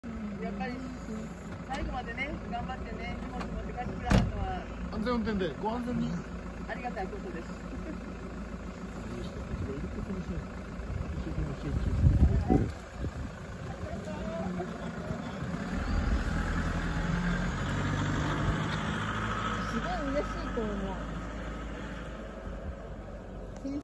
2024福ユ練試、荷物車見送。
2024/2/12、福井ユナイテッドFCは
練習試合でアトレチコ鈴鹿クラブ戦でした。